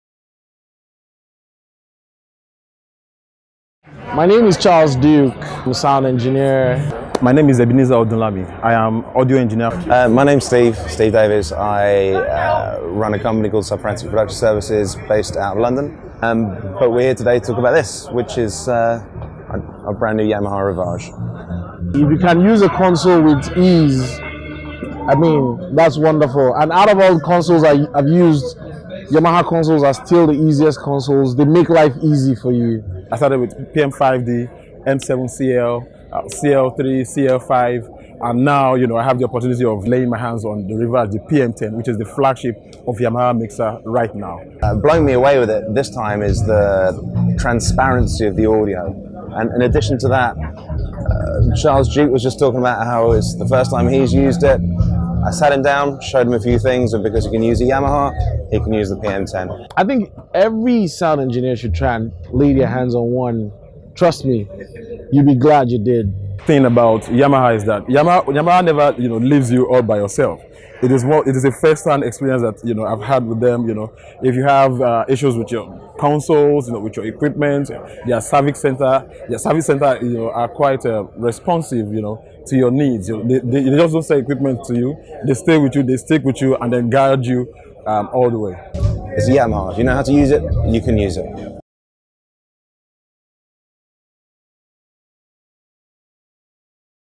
RIVAGE PM 10 at Experience 12 - Lagos 2017
Sound engineer's talk about their experience with Yamaha Rivage PM10 at “The Experience12 – Lagos”, which is one of the largest gospel musical concert in Africa.